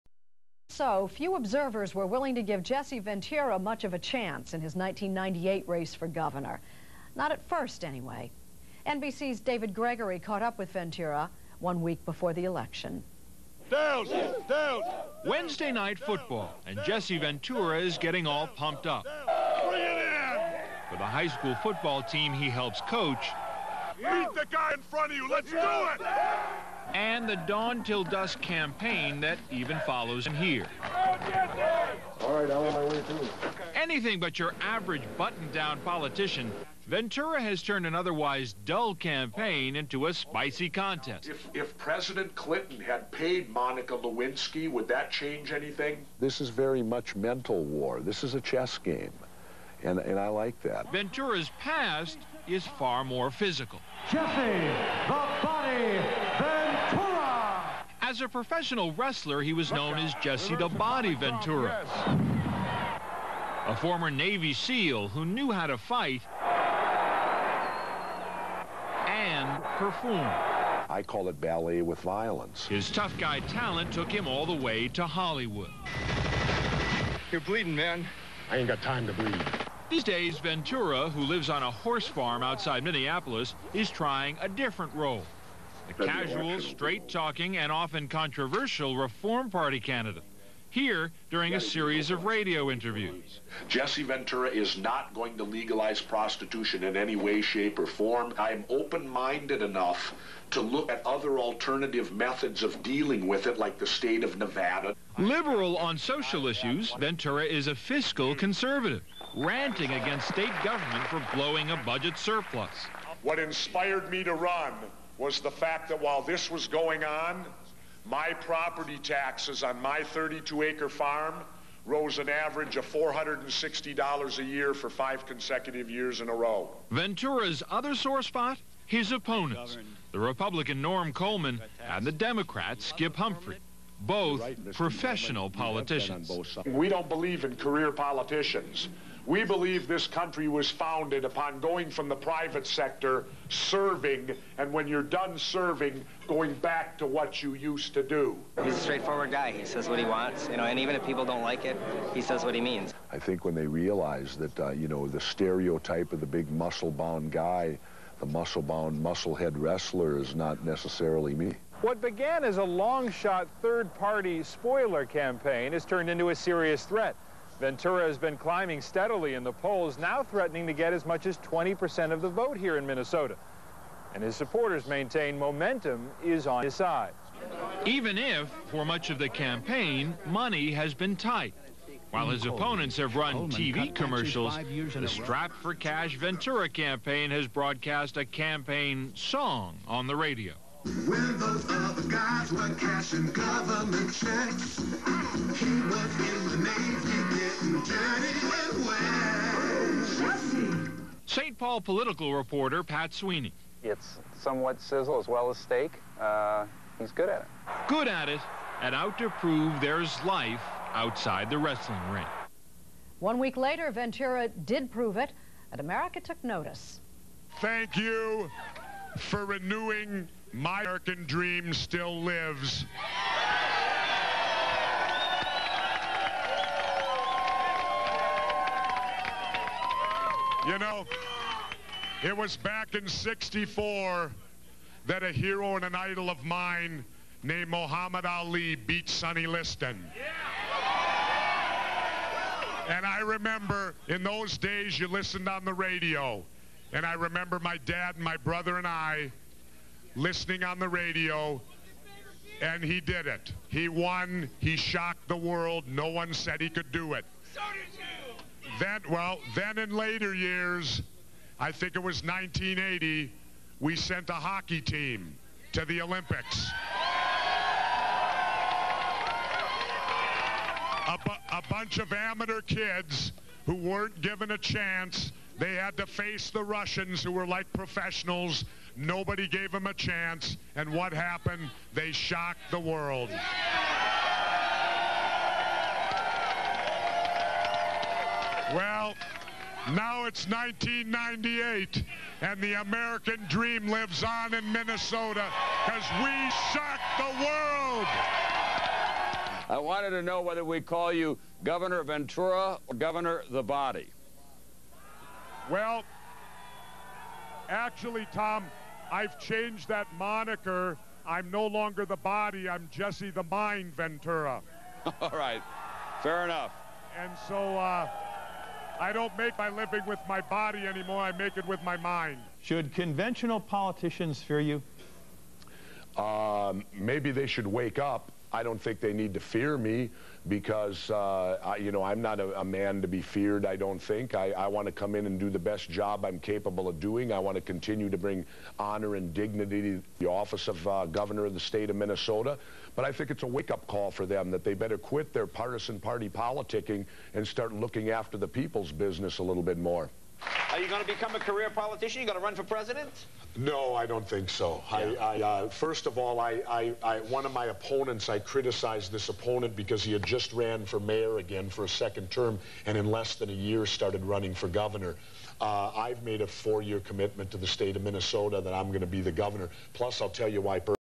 Jesse Ventura - Last Week Before Election 1998
The last week of the victorious election campaign of 1998. Jesse's opponents in the Governor's race Republican Norm Coleman and Democrat Skip Humphrey seemed to not realize the gathering storm of voter discontent with the two-party system that would soon sweep them into the dustbin of history.  Much like the oblivious leaders of the Democrat and Republican parties today.  Do they think the two-party dictatorship on the national level will last forever?  Just ask Skip Humphrey and Norm Coleman, it won't.